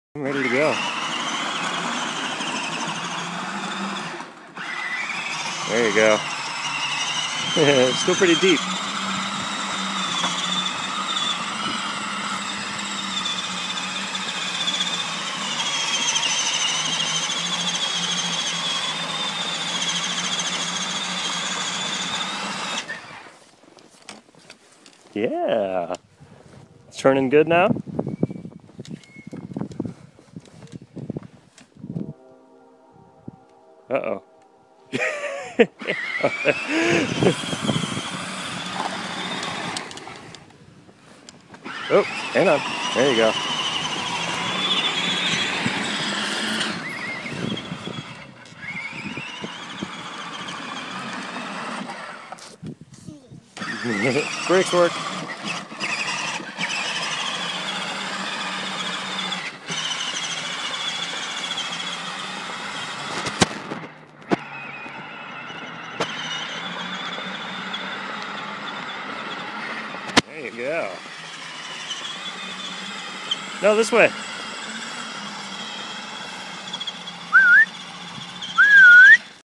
power wheels snowmobile test 2
Adding rudders to the skis greatly improved handling.
However, this test revealed a weakness in my wiring... back to the drawing board!